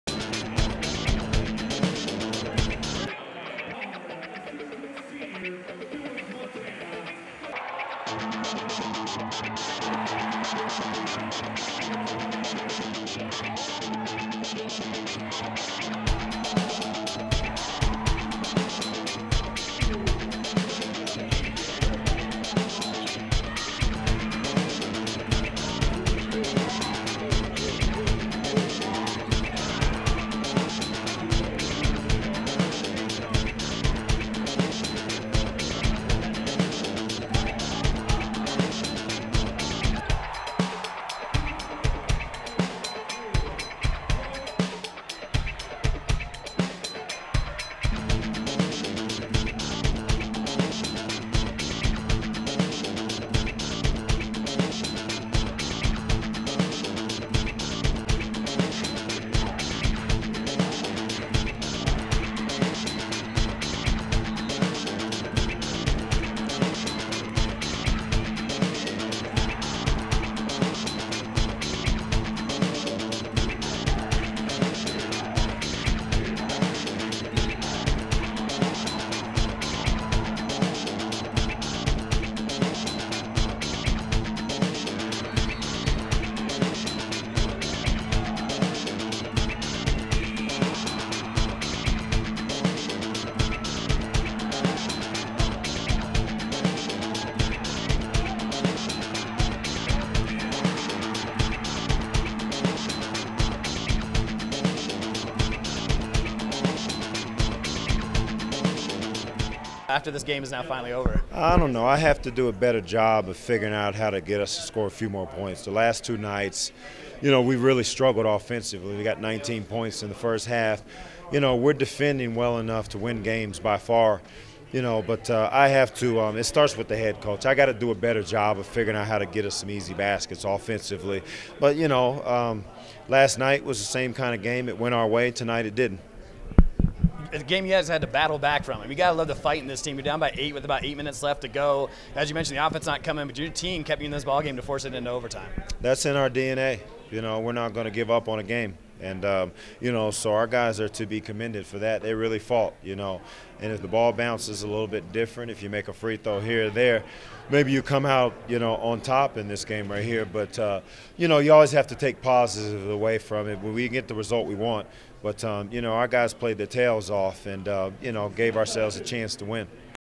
No. 10 Men's Basketball vs No. 9 Colorado Mines - Metro State
Mitch McCarron scored 27 points but Caleb Waitsman hits the game-winning basket right as the overtime buzzer expires to give Mines the 59-57. Post game interview with Head Coach Derrick Clark